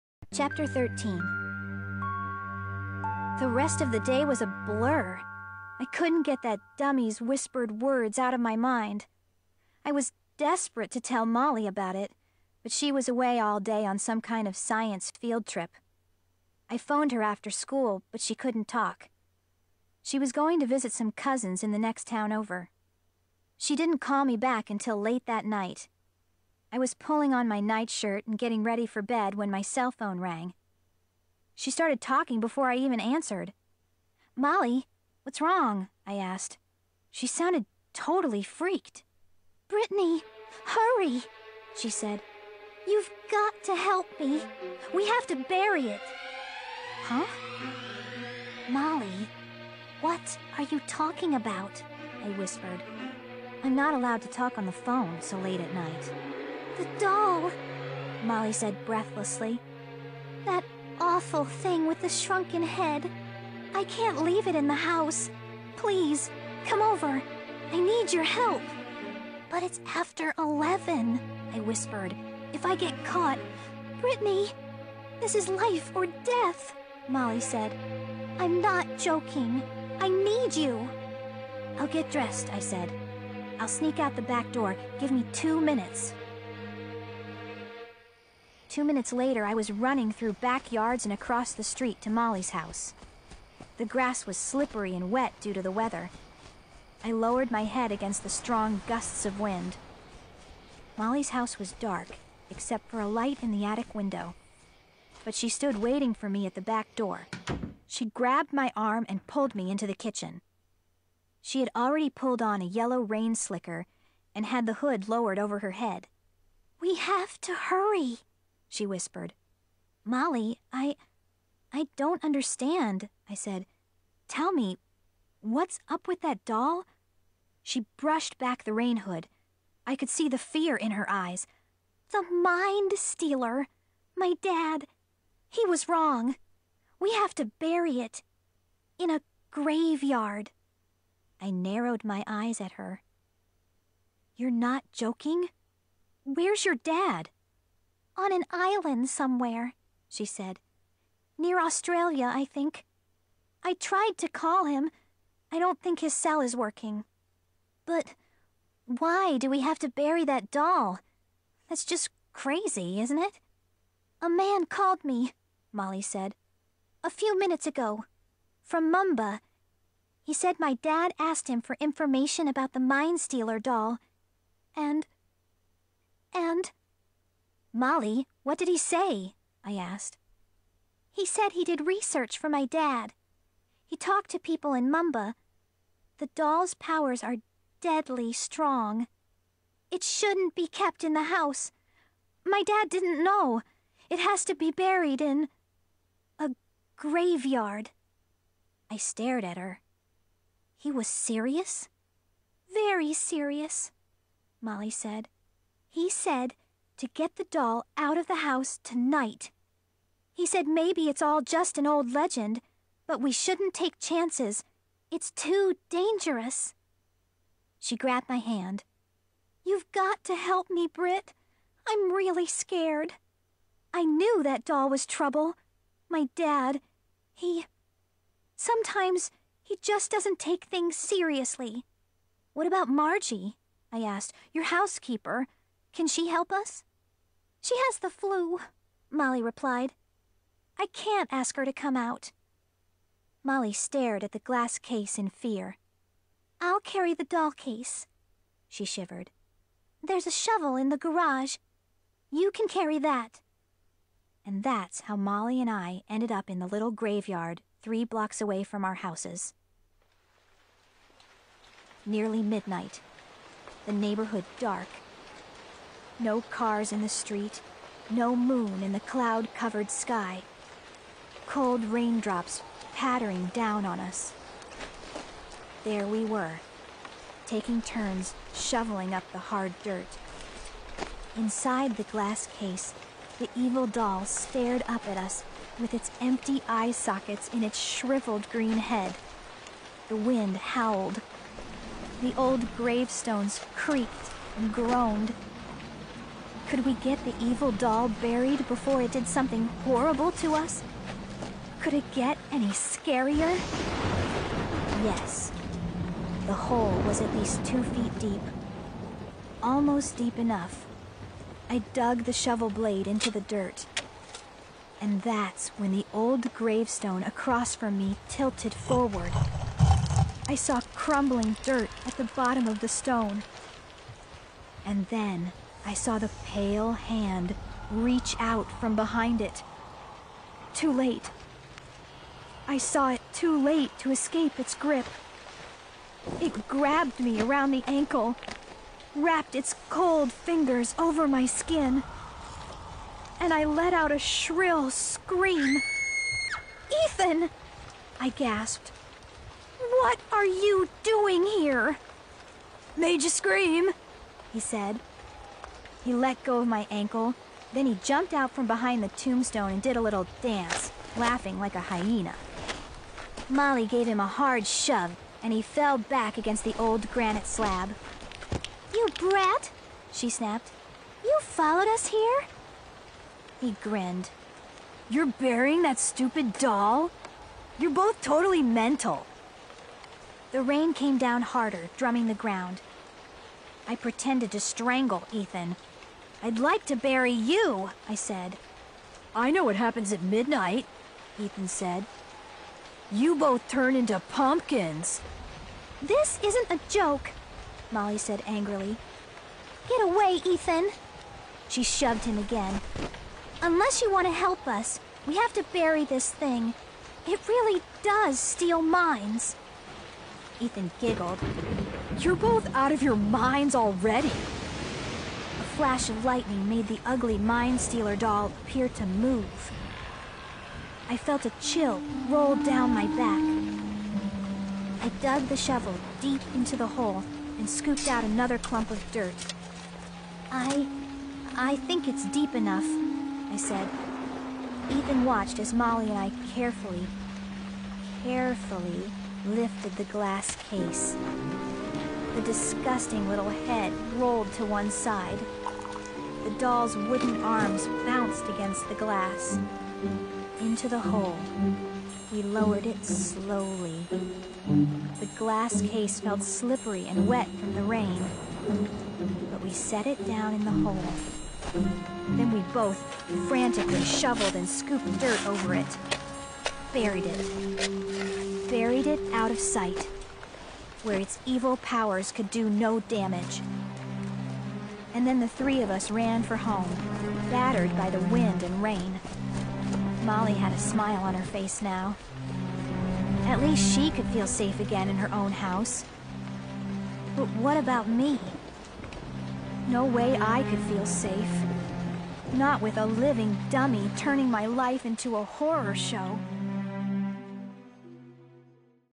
Goosebumps Horrorland #1 Revenge Of The Living Dummy (pt 13)
this is where Brittney and Molly left off in the graveyard to buried the evil Shrunken Head from the first Chapter. it all come back together as a puzzle